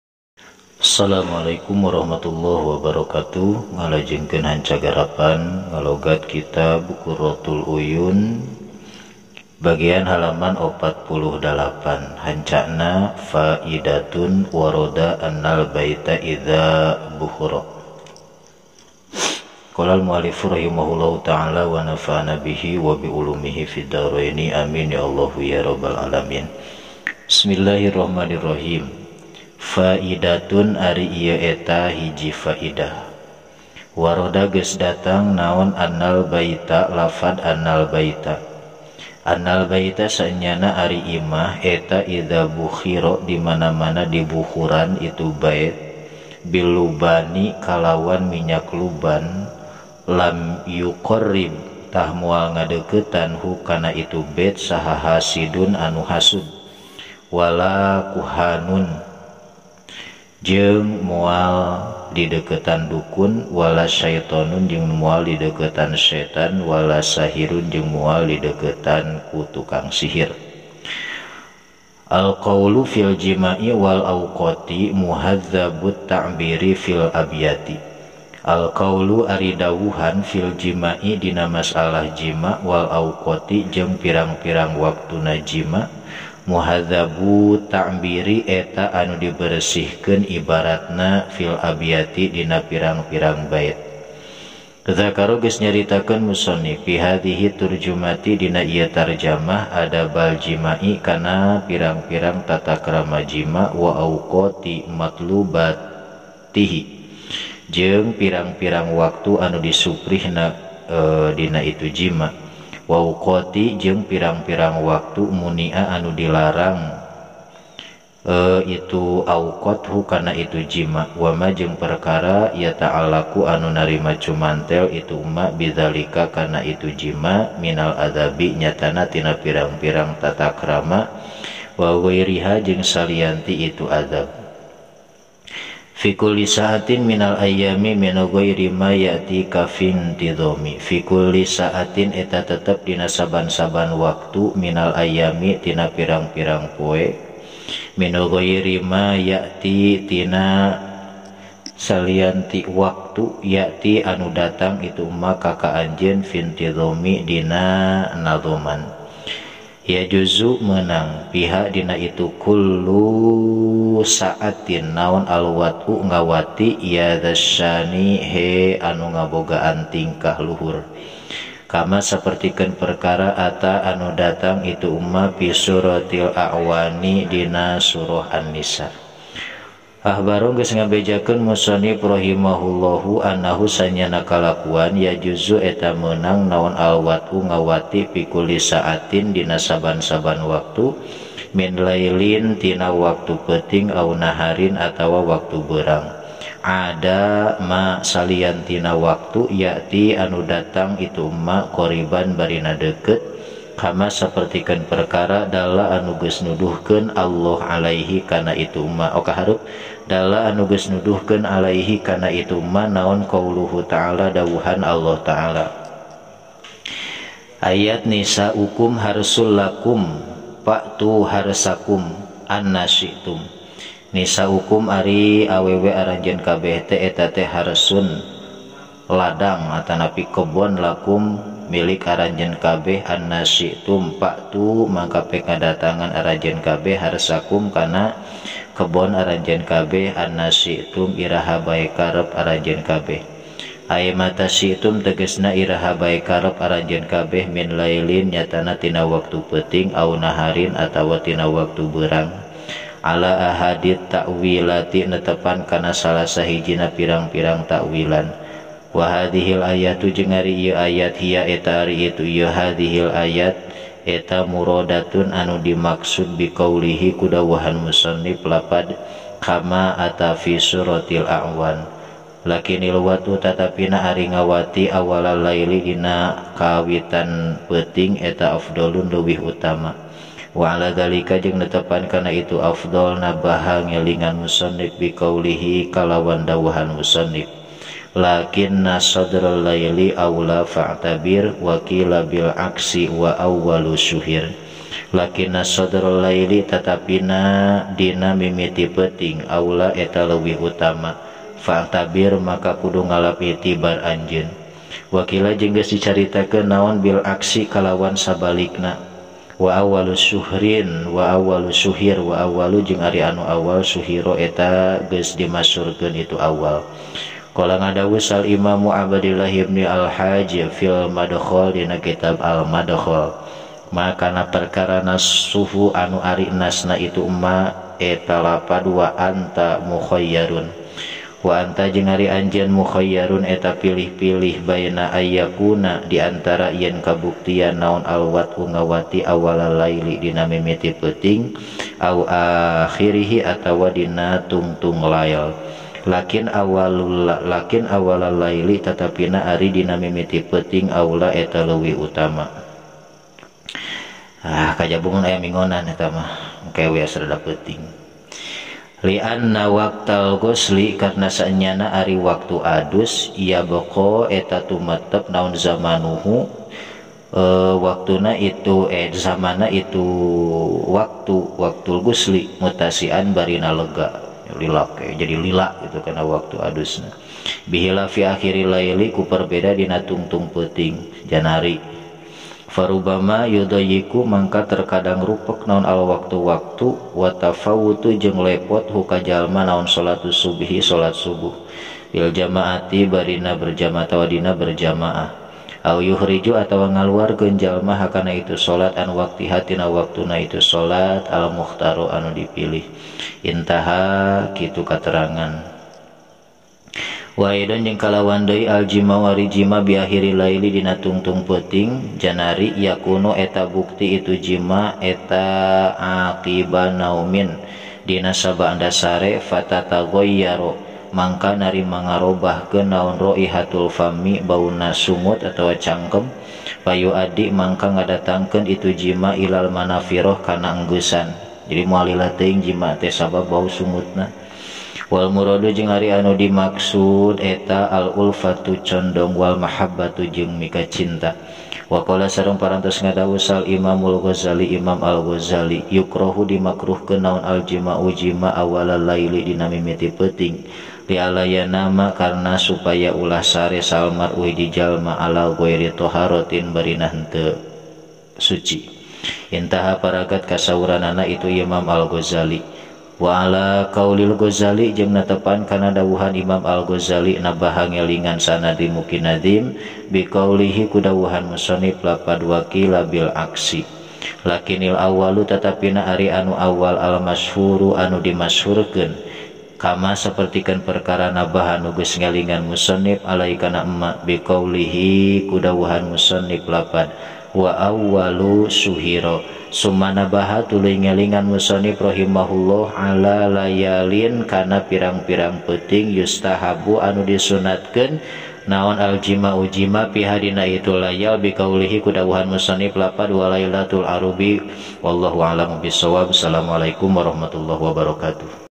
44. LUGOT SUNDA KITAB QURROTUL 'UYUN
Alhamdulillah atas rahmat, Hidayah dan inayah Alloh SWT,,, EMC dapat menerbitkan Lugot sunda kitab " QURROTUL 'UYUN "